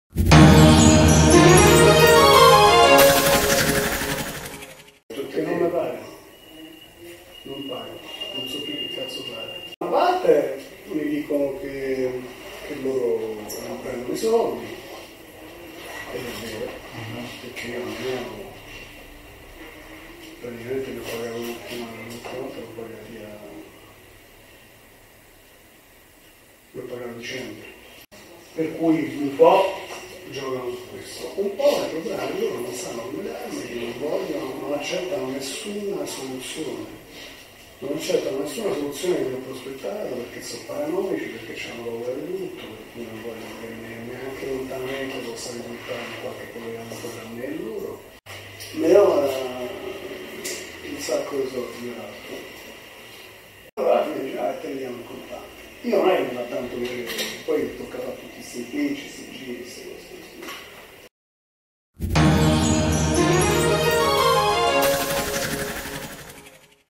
Mafia Capitale, Odevaine a Buzzi su La Cascina: "Se non mi pagano non so che c...fare"
“Non me pagano, non pagano, non so più che c… fare…da una parte mi dicono che loro non prendono i soldi… che l’ultima volta li hanno pagati a dicembre… per cui un po’ giocano su questo, un po’ il problema è che loro non sanno come darmeli, non vogliono, non accettano nessuna soluzione non accettano nessuna soluzione che gli ho prospettato… perché sono paranoici, perché c’hanno paura di tutto, perché non vogliono neanche lontanamente possa risulta qualche collegamento tra me e loro… mi devono dare un sacco di soldi… allora alla fine dice, te li diamo in contanti… io non è che non è che mi va tanto di… poi tocca fare tuti sti impicci, sti giri, ste cose…”. Così, nel corso di una conversazione intercettata Luca Odevaine si confronta con Salvatore Buzzi sulle difficoltà che stava incontrando a farsi dare il denaro da “La Cascina”.